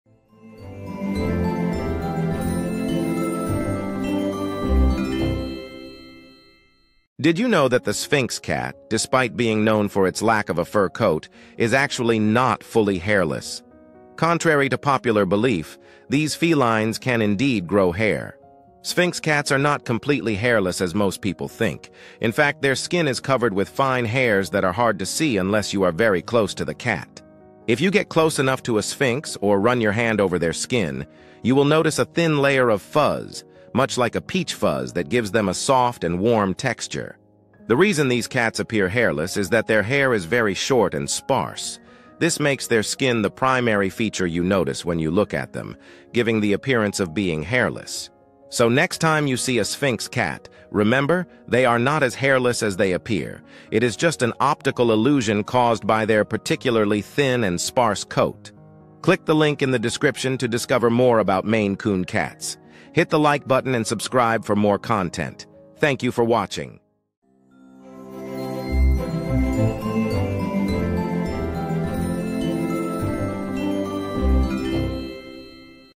Sphinx cat, despite being known for its lack of a fur coat, is actually not fully hairless?
Sphynx cats can actually grow a fine layer of peach-fuzz-like hair on their bodies. While they are known for their hairlessness, some Sphynx cats may develop a light covering of fur over time. This can vary from cat to cat and is a natural occurrence within the breed.